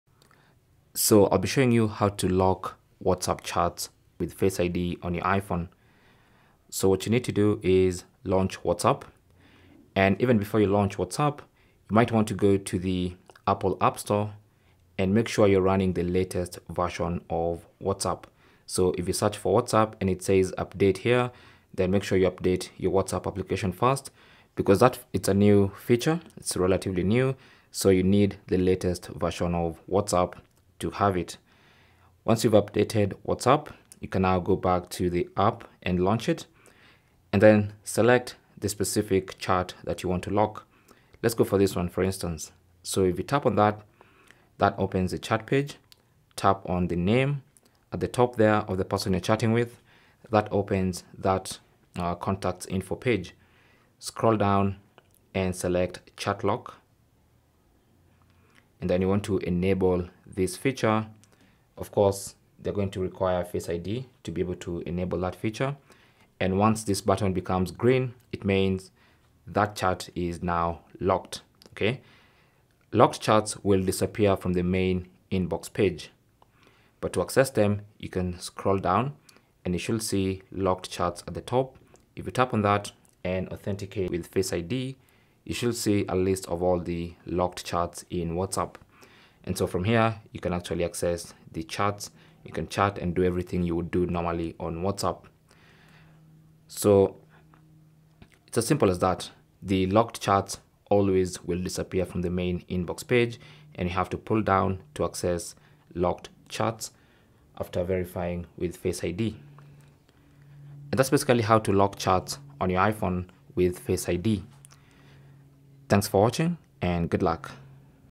How To Lock WhatsApp Chats With Face ID On iPhone
Learn how to lock WhatsApp chats with face ID on iPhone.

Face ID is a convenient and secure way to lock your private conversations on WhatsApp, ensuring that only you can access them. Whether you're concerned about your privacy or simply want an extra layer of protection for your sensitive messages, this tutorial will help you set up a Face ID lock for your WhatsApp chats.

Keep in mind that this is a new WhatsApp feature and you can only access it if you have the latest WhatsApp update. If not, update the app from the App Store to access the latest features and improvements.

To lock WhatsApp chats with Face ID on iPhone;
Update the application to the latest version
Launch the application
Find the conversation you'd like to lock and tap on it.
Tap on the toggle icon next to "Lock This Chat with Face ID" to lock the WhatsApp chat
If prompted, use your Face ID to successfully make the changes.

Galaxy S23 Ultra DUAL SIM 256GB 8GB Phantom Black:

Samsung Galaxy A14 (Factory Unlocked):

Cell Phone Tripod Adapter